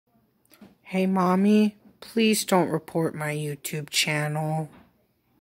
Message To My Mom
hey guys I'm afraid that my mom's going to report my YouTube channel but I don't want that to happen so please tell my mom don't report my YouTube channel I am the best YouTuber and I only have four more subscribers left until I get the community Tab and I will talk to people in the community on YouTube